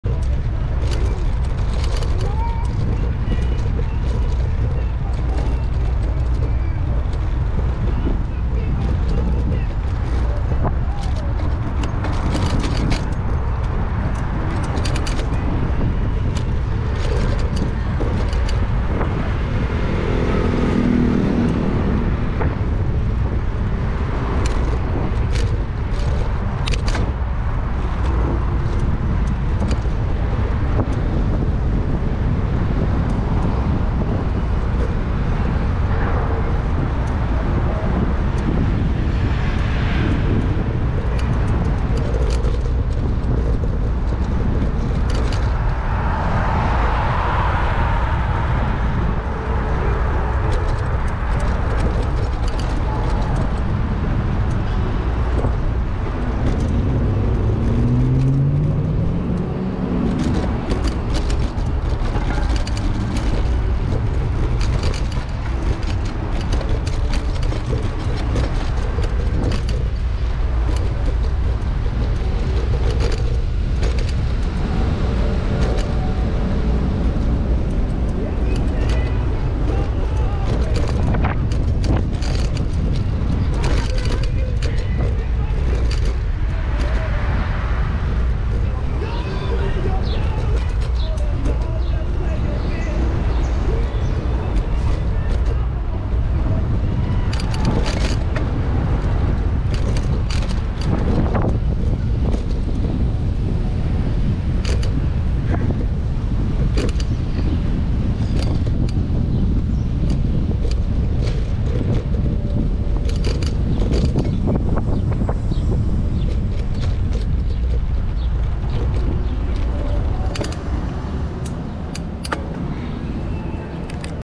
Bike from from the Margaret Pace Park, Downtown, Miami, Brickell and Key Biscayne in Miami Florida.8
Bike from from the Margaret Pace Park, Downtown, Miami, Brickell and then to Key Biscayne areas of Miami Florida.

Ever thought about making your own video's?